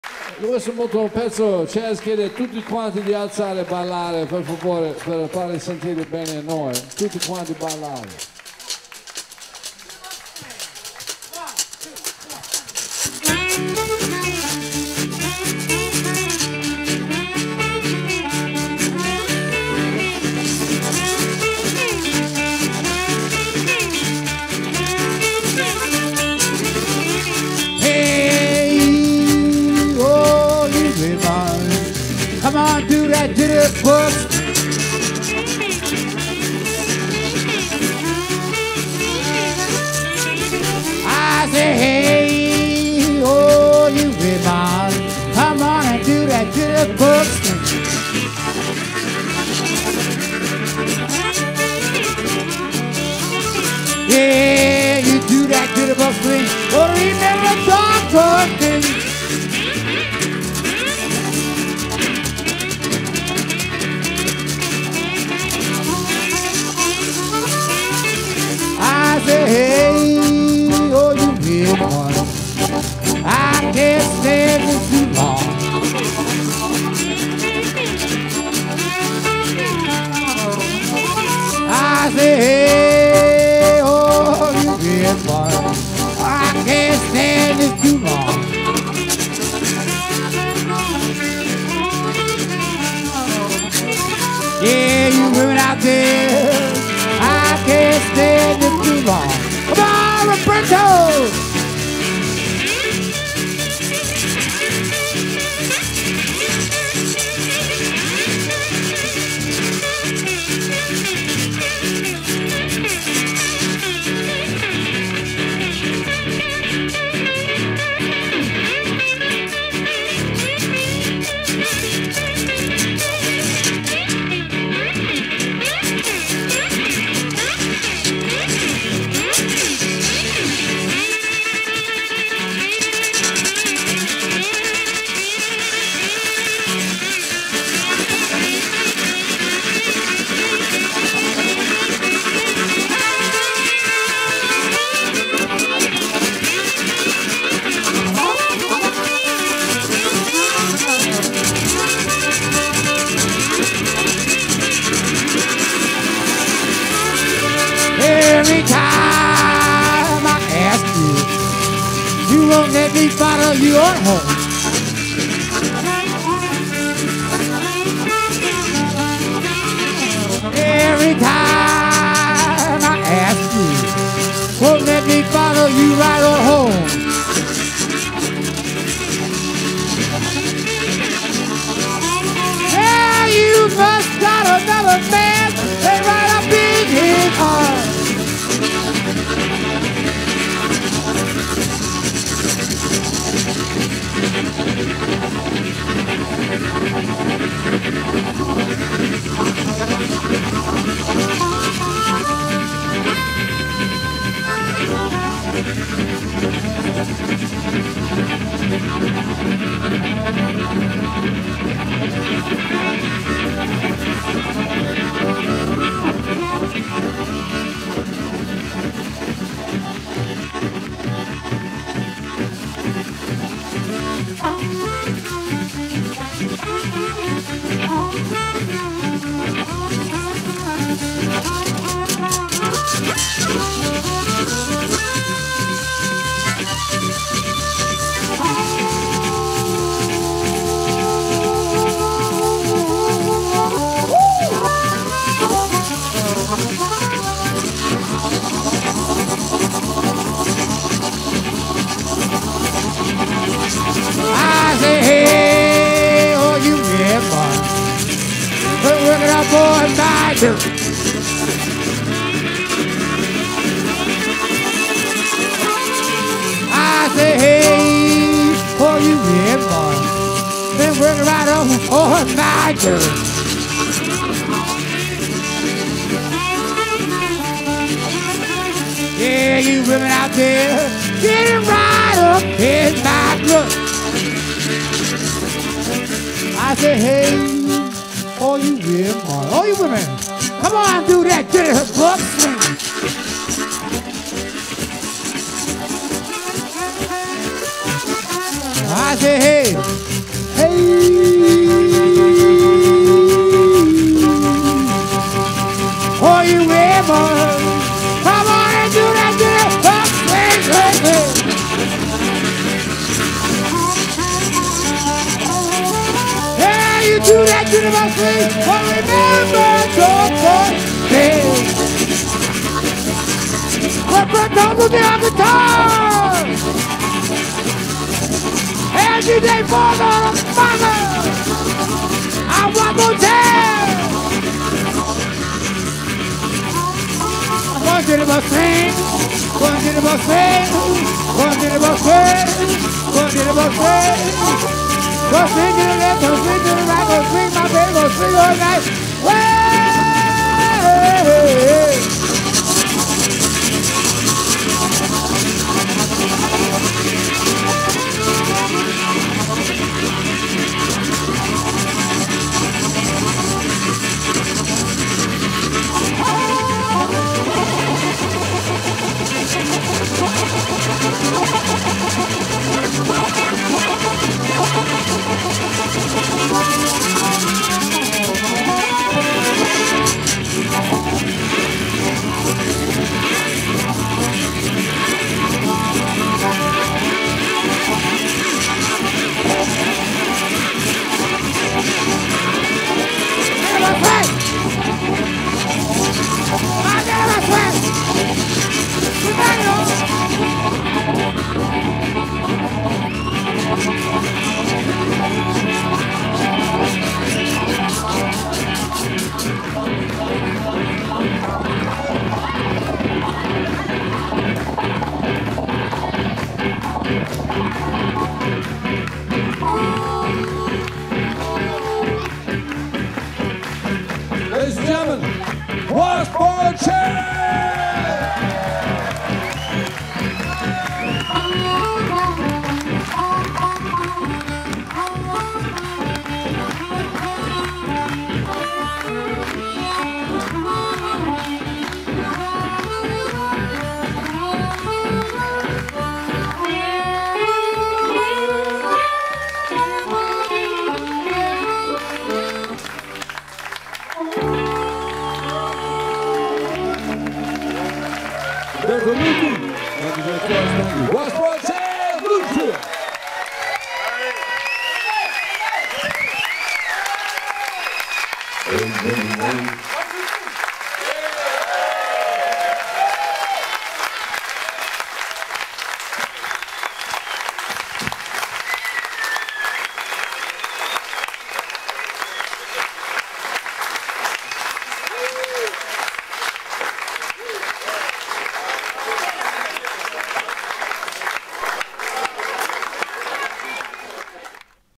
Washboard Chaz Trio - Brivio (I) 2010 - End of the gig
27 Giugno 2010 - Brivio (Italy) - Italian Blues River Festival
Washboard Chaz Trio - This is the last song of the gig
Washboard Chaz (washboard, voc), Andy J. Forest (harp, voc), Roberto Luti (gtr)

Audio: Altr'onde - Video & Editing: Dario Guidotti (blueCacao)